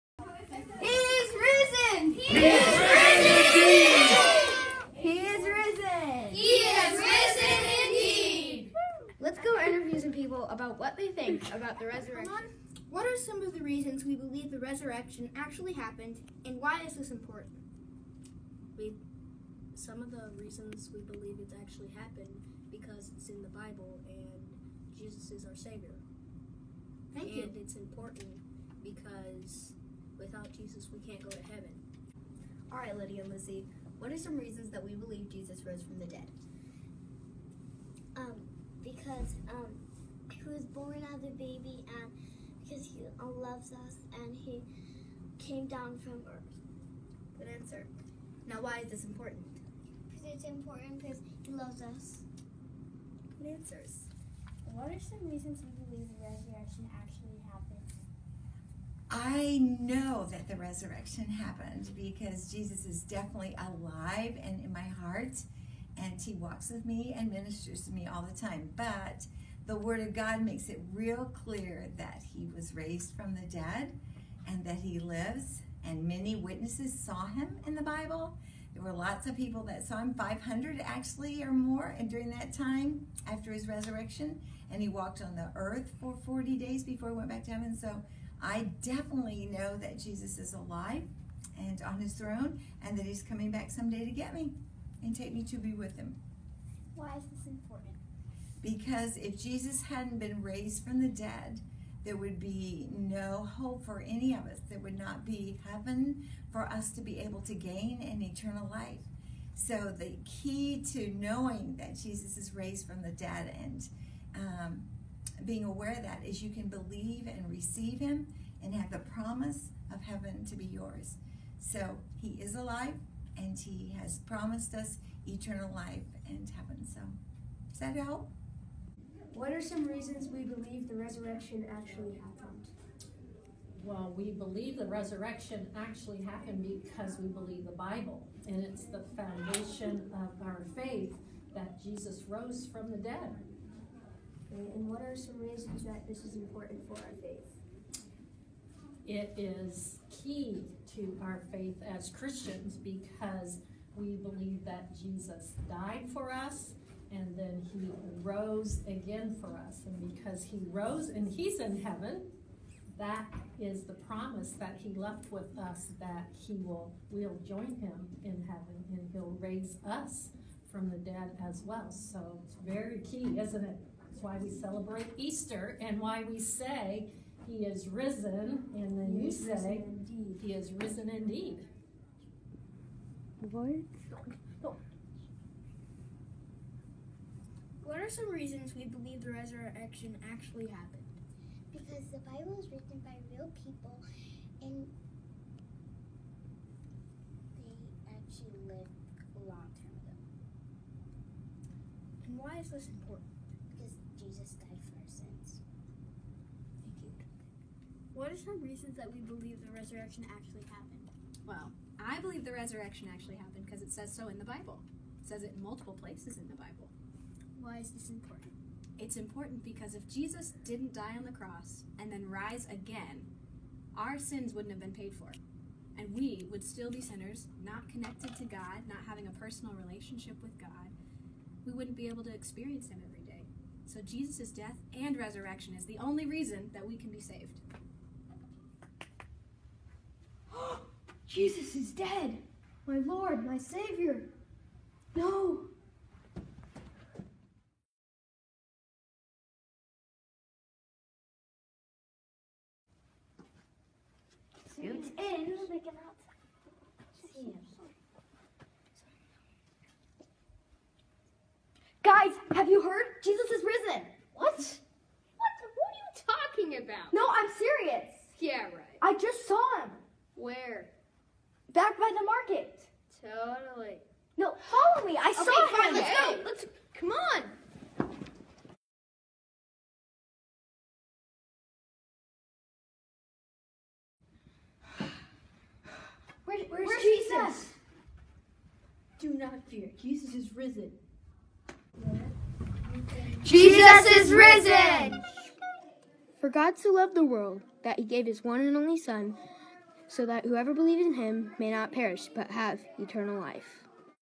Heritage Christian School 4th Graders Explain Easter
Our 4th graders put together this video for Easter! It was shown to the entire elementary school earlier this week at Chapel. Praise the Lord for their faith and joy in sharing the Good News! He is Risen!